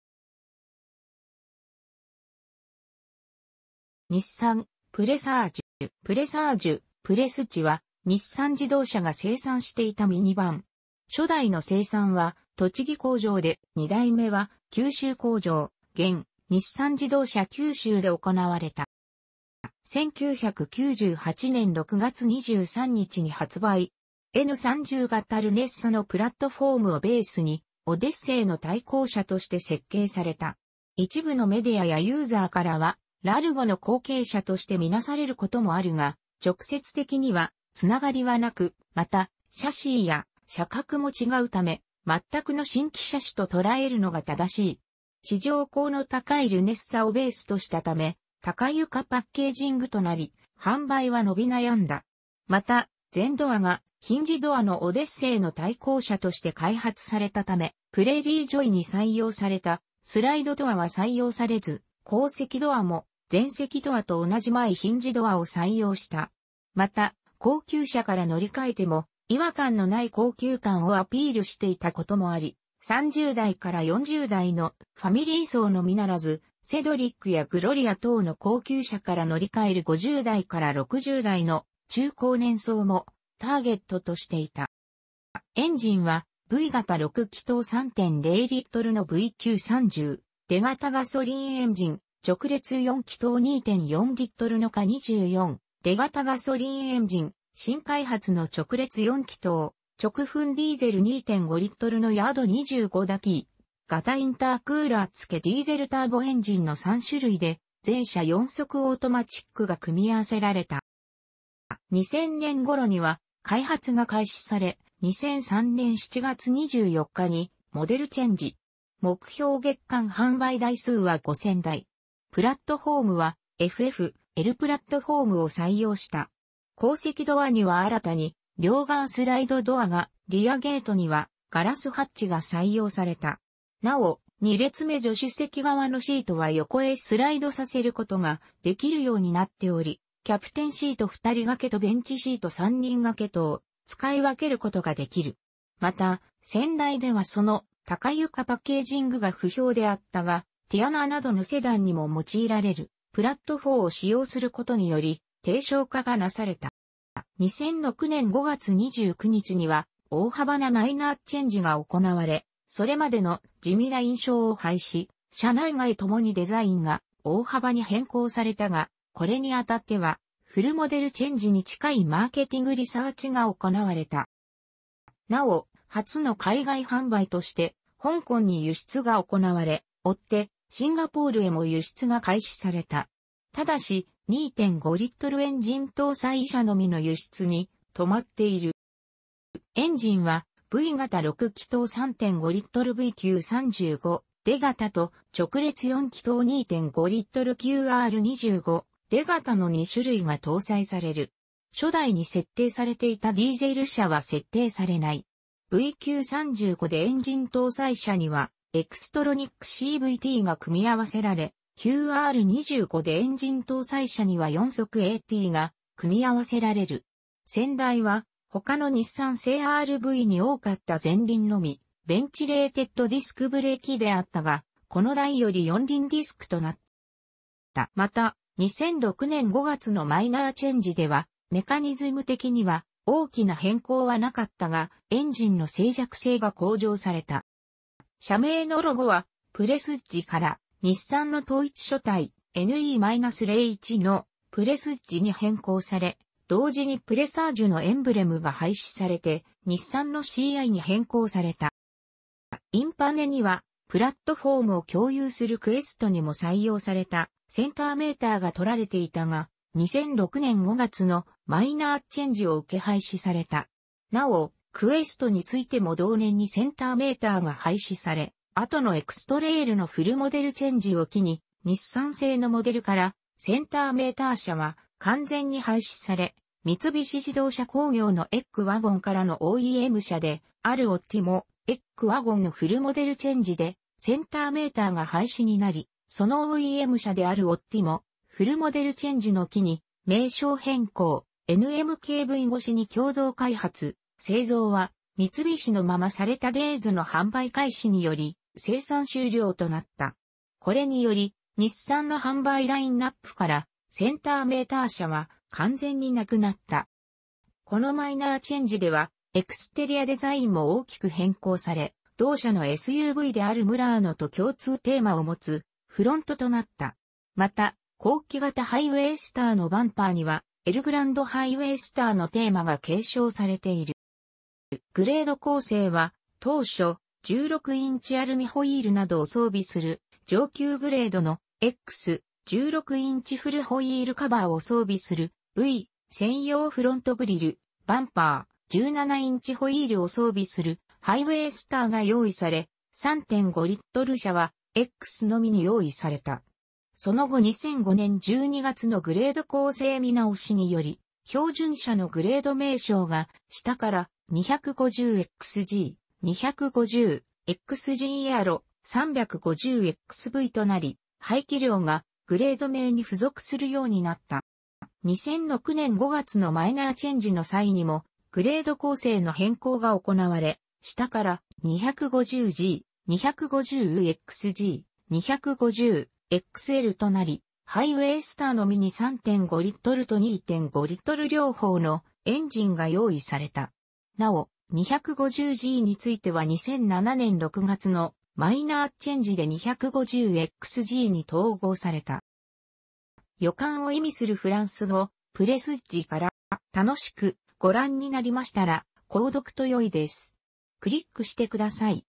日産・プレサージュ
プレサージュ（"PRESAGE" ）は、日産自動車が生産していたミニバン。初代の生産は栃木工場で、2代目は九州工場（現・日産自動車九州）で行われた。

1998年6月23日に発売。N30型ルネッサのプラットフォームをベースにオデッセイの対抗車として設計された。一部のメディアやユーザーからはラルゴの後継車として見做される事もあるが、直接的には繋がりは無く、又、シャシーや車格も違う為、全くの新規車種と捉えるのが正しい。地上高の高いルネッサをベースとしたため、高床パッケージングとなり、販売は伸び悩んだ。また、全ドアがヒンジドアのオデッセイの対抗車として開発されたため、プレーリージョイに採用されたスライドドアは採用されず、後席ドアも前席ドアと同じ前ヒンジドアを採用した。又、「高級車から乗り換えても違和感の無い高級感」をアピールしていた事もあり、30代から40代のファミリー層のみならず、セドリックやグロリア等の高級車から乗り換える50代から60代の中高年層もターゲットとしていた。

エンジンは、V型6気筒3.0LのVQ30DE型ガソリンエンジン、直列4気筒2.4LのKA24DE型ガソリンエンジン、新開発の直列4気筒・直噴ディーゼル2.5LのYD25DDTi型インタークーラー付ディーゼルターボエンジンの3種類で、全車4速オートマチックが組み合わせられた。

2000年ごろには開発が開始され、2003年7月24日にモデルチェンジ。目標月間販売台数は5000台。プラットフォームはFF-Lプラットフォームを採用した。後席ドアには新たに両側スライドドアが、リヤゲートにはガラスハッチが採用された。なお、2列目助手席側のシートは横へスライドさせることができるようになっており、キャプテンシート2人掛けとベンチシート3人掛けとを使い分けることができる。また、先代ではその高床パッケージングが不評であったが、ティアナなどのセダンにも用いられるプラットフォームを使用することにより低床化がなされた。

2006年5月29日には大幅なマイナーチェンジが行われ、それまでの「地味」な印象を廃し、車内外ともにデザインが大幅に変更されたが、これに当たってはフルモデルチェンジに近いマーケティングリサーチが行われた。

なお、初の海外販売として香港に輸出が行われ、追ってシンガポールへも輸出が開始された。ただし、2.5Lエンジン搭載車のみの輸出に留まっている。

エンジンはV型6気筒 3.5L VQ35DE型と、直列4気筒 2.5L QR25DE型の2種類が搭載される。初代に設定されていたディーゼル車は設定されない。VQ35DEエンジン搭載車には、エクストロニックCVTが組み合わせられ、QR25DEエンジン搭載車には4速ATが組み合わせられる。先代は他の日産製RVに多かった前輪のみベンチレーテッドディスクブレーキであったがこの代より4輪ディスクとなった。

また、2006年5月のマイナーチェンジでは、メカニズム的には大きな変更はなかったが、エンジンの静寂性が向上された。

車名のロゴは「Presage」から日産の統一書体、NE-01の「PRESAGE」に変更され、同時にプレサージュのエンブレムが廃止されて日産のCIに変更された。

インパネには、プラットフォームを共有するクエストにも採用されたセンターメーターがとられていたが、2006年5月のマイナーチェンジを受け廃止された。なお、クエストについても同年にセンターメーターが廃止され、後のエクストレイルのフルモデルチェンジを機に日産製のモデルからセンターメーター車は完全に廃止され、三菱自動車工業のeKワゴンからのOEM車であるオッティも、eKワゴンのフルモデルチェンジでセンターメーターが廃止になり、そのOEM車であるオッティもフルモデルチェンジの機に名称変更・NMKV越しに共同開発（製造は三菱のまま）されたデイズの販売開始により生産終了となった。これにより、日産の販売ラインナップからセンターメーター車は完全に無くなった。

このマイナーチェンジではエクステリアデザインも大きく変更され、同社のSUVであるムラーノと共通テーマを持つフロントとなった。また、後期型ハイウェイスターのバンパーにはエルグランドハイウェイスターのテーマが継承されている。

グレード構成は当初、16インチアルミホイールなどを装備する上級グレードの「X」、16インチフルホイールカバーを装備する「V」、専用フロントグ...